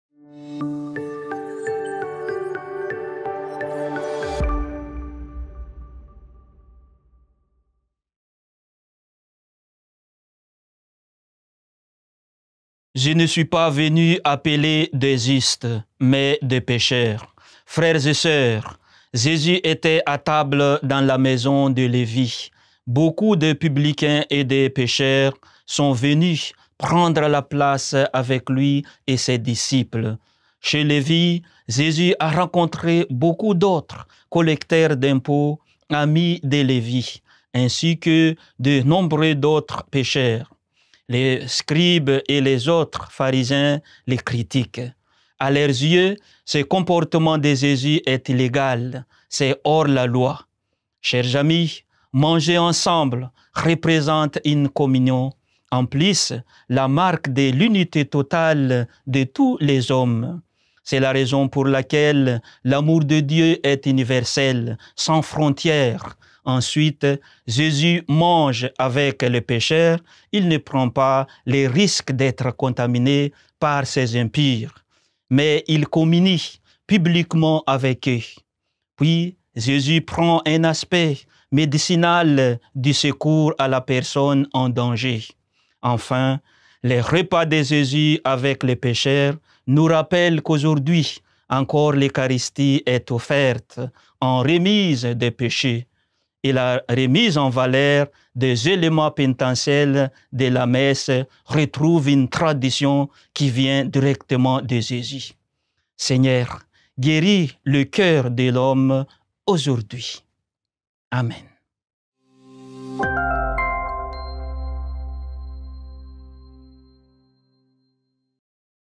[ 𝗟𝗮 𝗠𝗶𝗻𝘂𝘁𝗲 𝗦𝗽𝗶 🙏 ] samedi 13 janvier - Marc 2, 13-17
« Je ne suis pas venu appeler des justes, mais des pécheurs »

Samedi 13 janvier - Marc 2, 13-17 avec le père Marcel RAMISY, chapelain au sanctuaire diocésain Notre-Dame-de-Laghet.